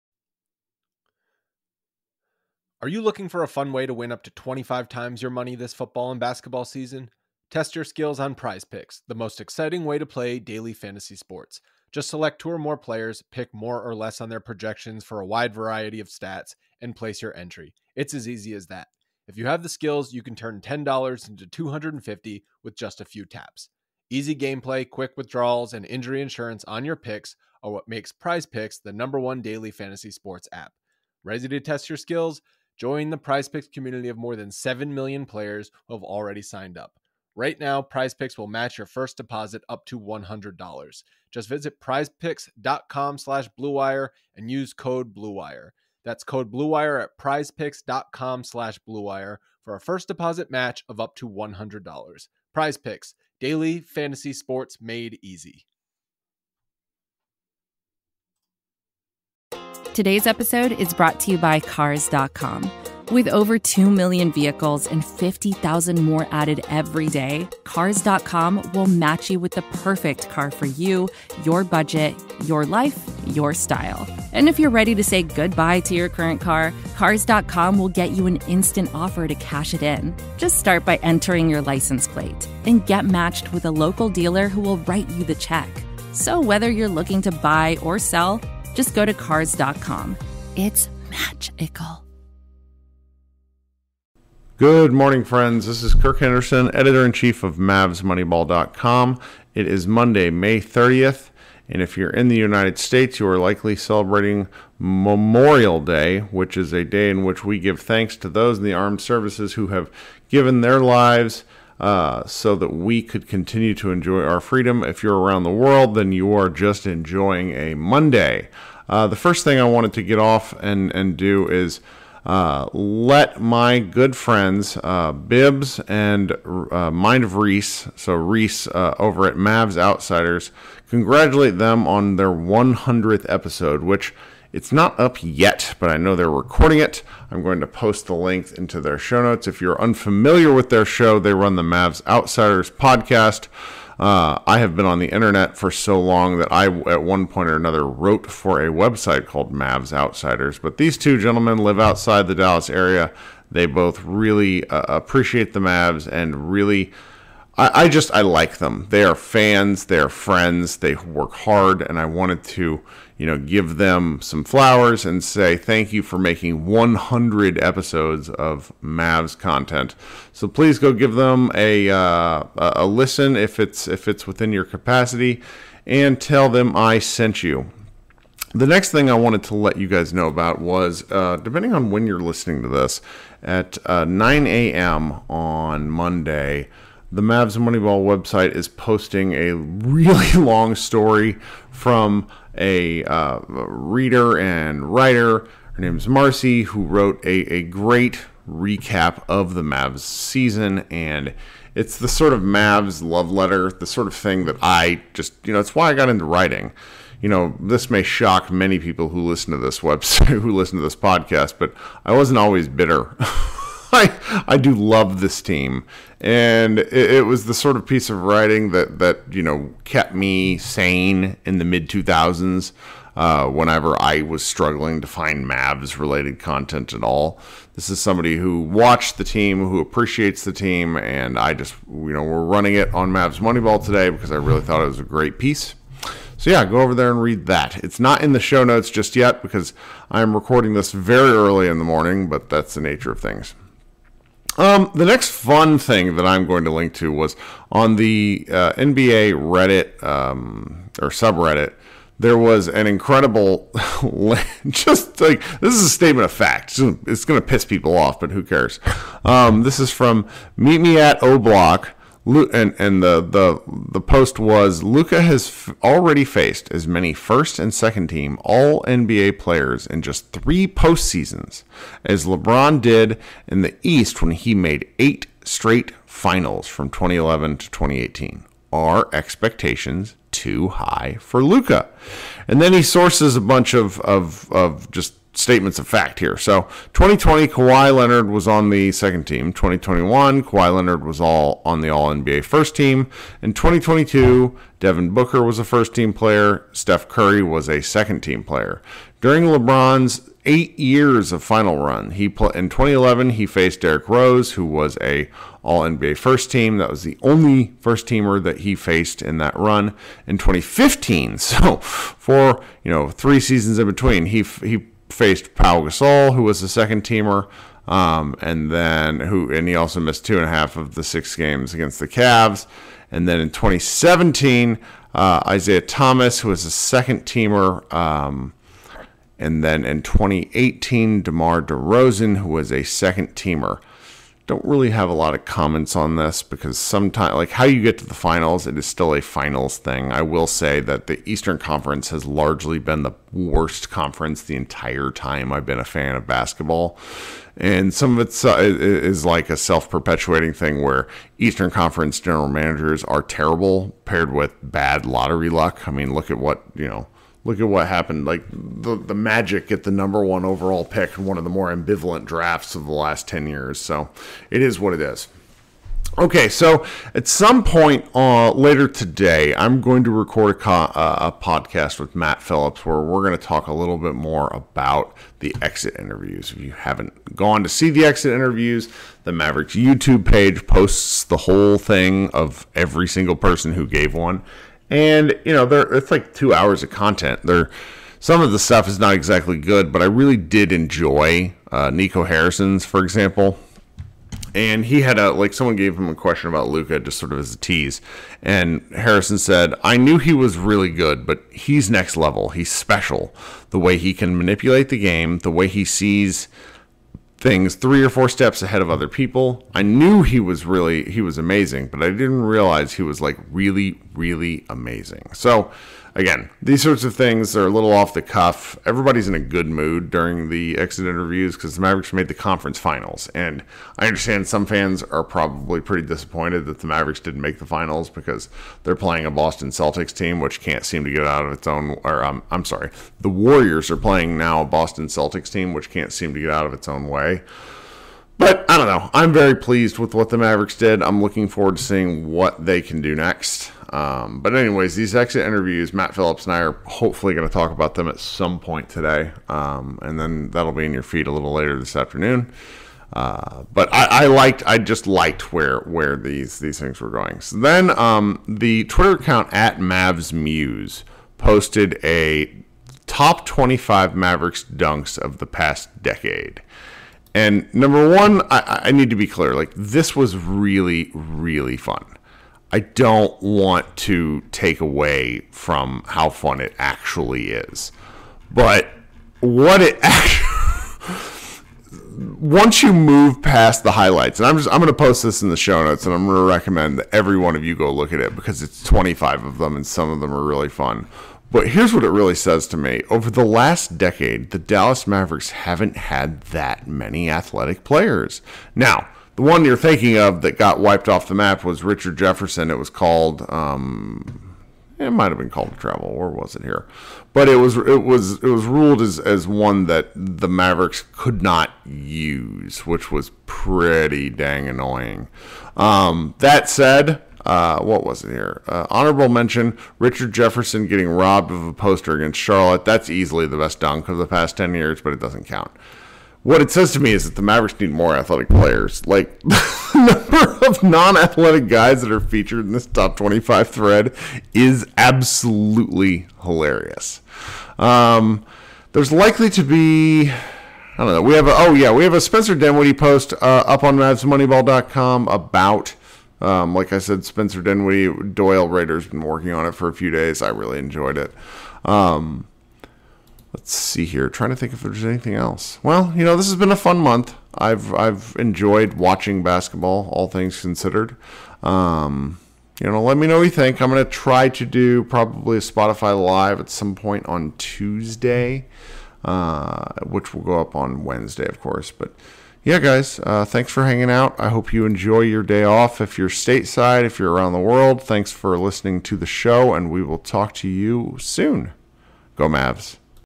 Moneyball Minute: 100th episode for friends of the show Mavs Outsiders, Luka's path vs Lebron's, ...
First, Kirk points you to friends of the show Mavs Outsiders, who are celebrating their 100th episode soon. Click here for Apple downloads, here for Spotify. Then there's a fun Reddit post comparing Luka Doncic;'s playoff paths versus that of Lebron James. Next there's mention of Nico Harrison's exit interview. Last there's Mavs Muse fun thread on Dallas dunks the last decade.
Learn more about your ad choices.